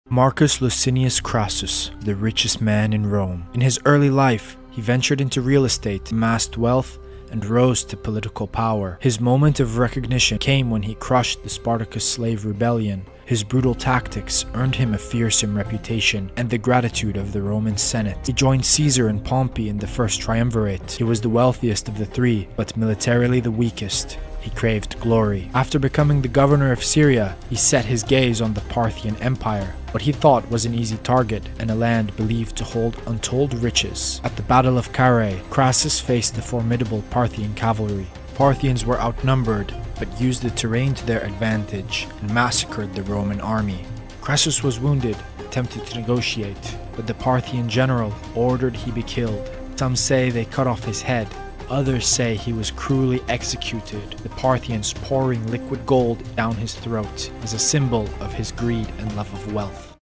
Execution by GOLD: The Fate of Marcus Crassus, Rome's Wealthiest Man
The Rise and Fall of Crassus | 60-Second History Short
Witness Marcus Crassus journey from wealth accumulation to the infamous Battle of Carrhae, a turning point in Roman history.